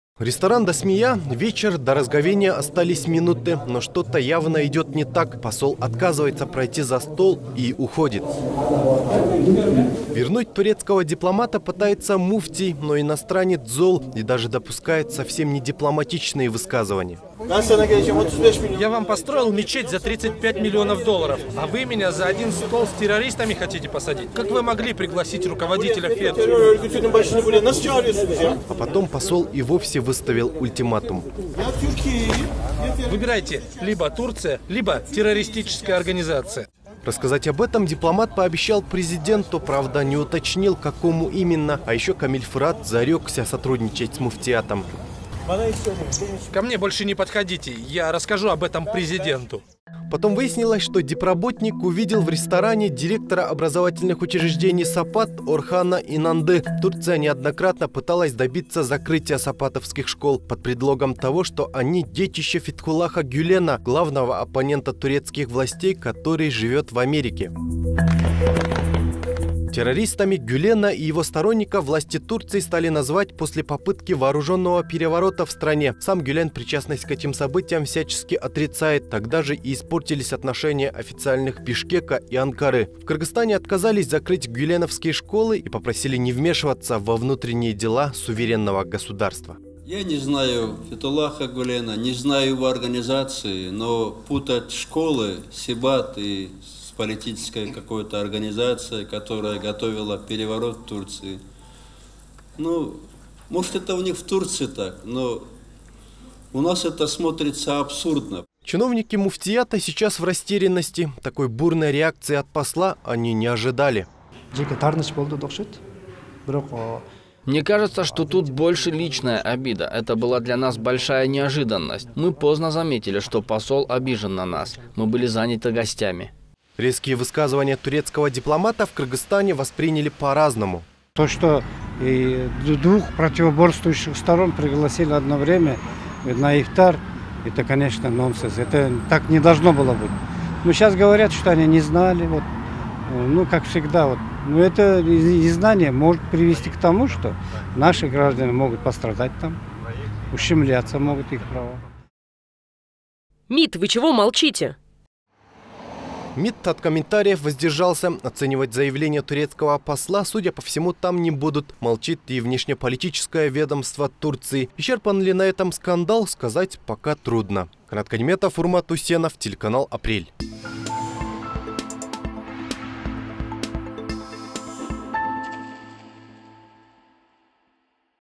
ПОСОЛ Турции УГРОЖАЛ пожаловаться ЖЭЭНБЕКОВУ на МУФТИЯТ \\ 13.05.2019 \\ Апрель ТВ
Посол Турции Женгиз Камил Фырат и представили движения Гюлена оказались в Бишкеке за одним столом во время ифтара. Обернулось всё грандиозным скандалом. Дипломат демонстративно покинул ресторан, в котором проходило мероприятие.